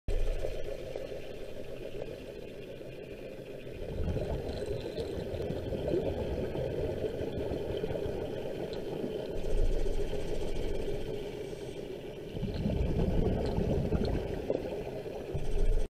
Quick Mask Try-Out
Did a 30 minute dive today to test my new OTS Spectrum and do my self required complete minimum yearly basic skills run through. Had a blast in the 68F water.